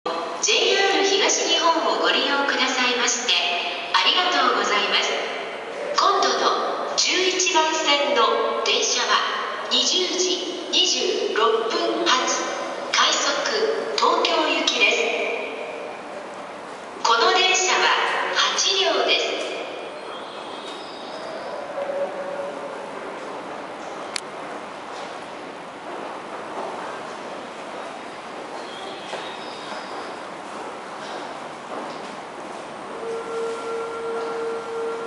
武蔵野線 西船橋駅 ATOS使用開始初日 11番線
2012年1月22日に使用開始となりました。
まだPRCタイプの表示です。また、この日限りで聞き納めとなった「ATOS放送」も入っています。